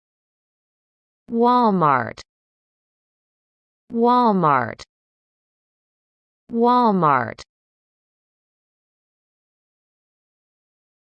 How to pronounce Walmart | Walmart pronunciation in American English
This video will help you to pronounce Walmart in American English. Hope you like it 😄